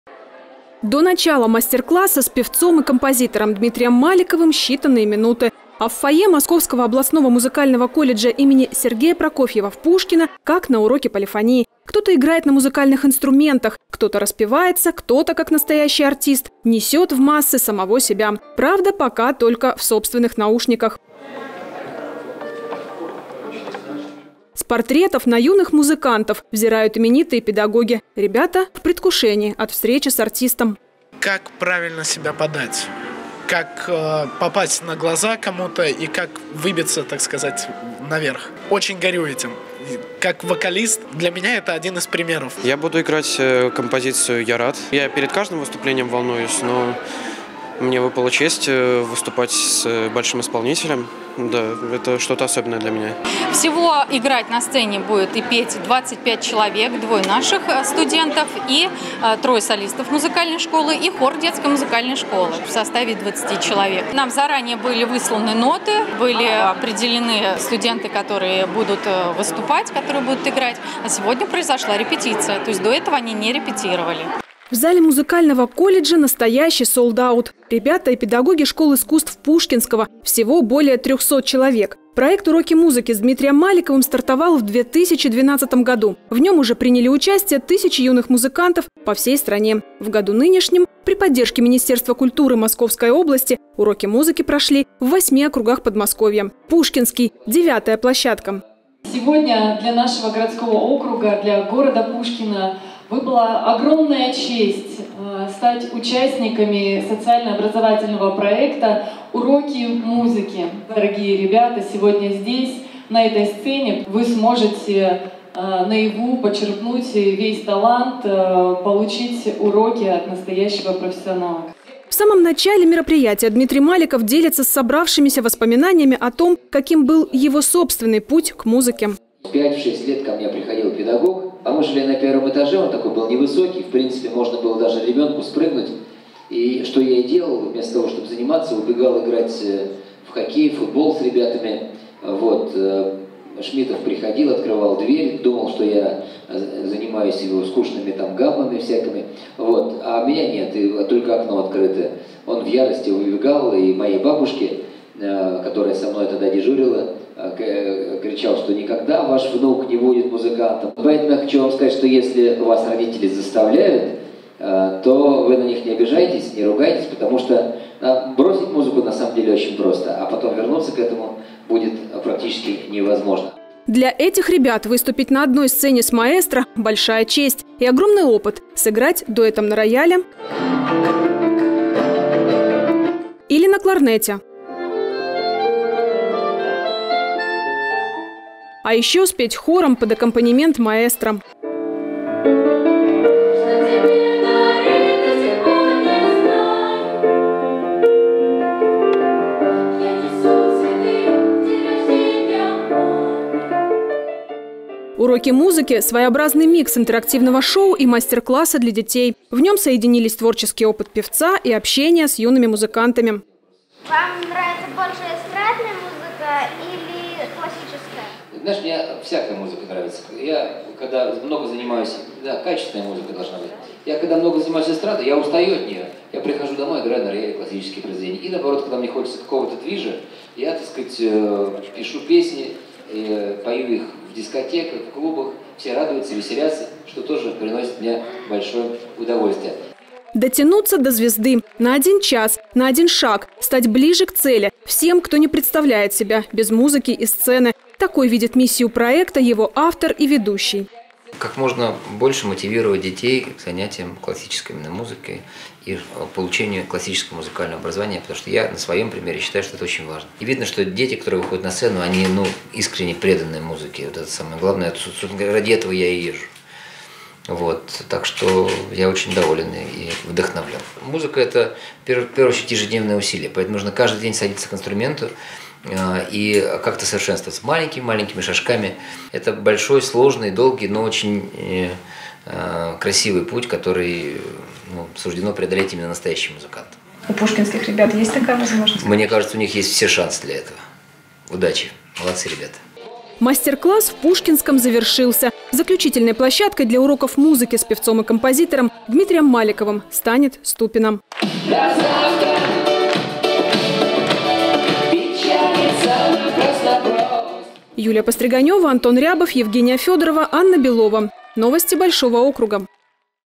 Дмитрий Маликов дал мастер-класс студентам музыкального колледжа имени С.С. Прокофьева
Связанные любовью к музыке. Воспитанники детских школ искусств Пушкинского на этой неделе получили приятный сюрприз. В наш округ приехал народный артист России Дмитрий Маликов. Маэстро провёл для ребят «Уроки музыки». Это социальный проект, целью которого является поддержка детского музыкального образования в стране. Встреча прошла в Московском областном музыкальном колледже имени Прокофьева.